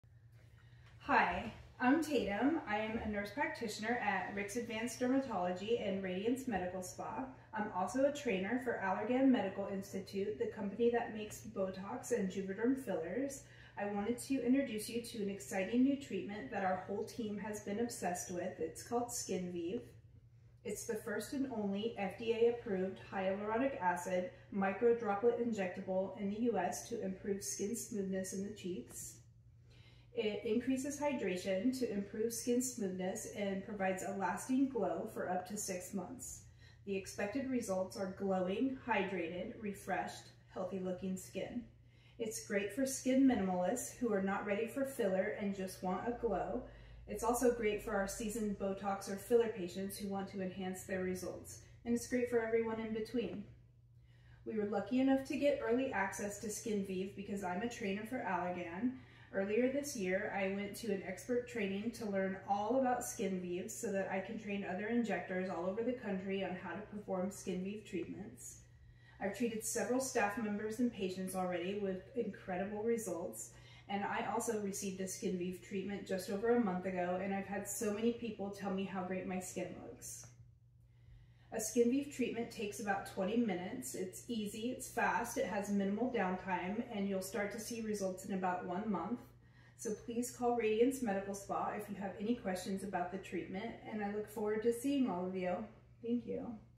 Introducing SkinVive!
Tatum Toland, APRN discusses SkinVive! It is the first and only FDA-approved hyaluronic acid microdroplet injectable in the US to improve skin smoothness of the cheeks. It increases hydration to improve skin smoothness and provide a lasting glow for up to 6 months! Expected results: glowing, hydrated, refreshed, healthy-looking skin. Fast, easy treatment with minimal downtime!